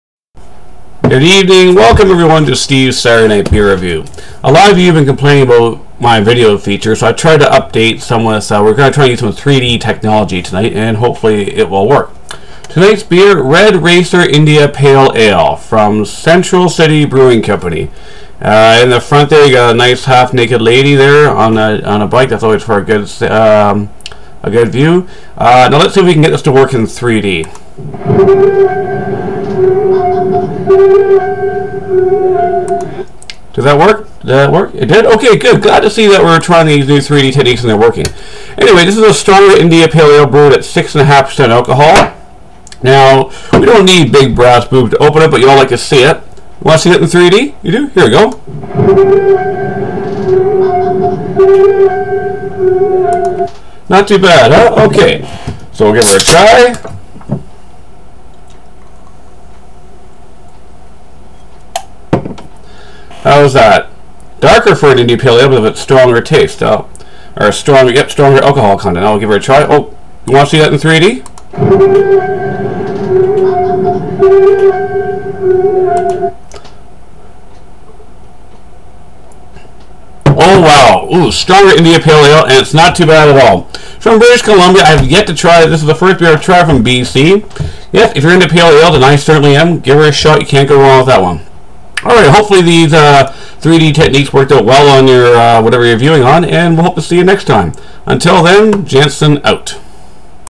Steve's Saturday Night Beer Review Episode 51
Red Racer IPA(Central City Brewing, British Colombia Canada)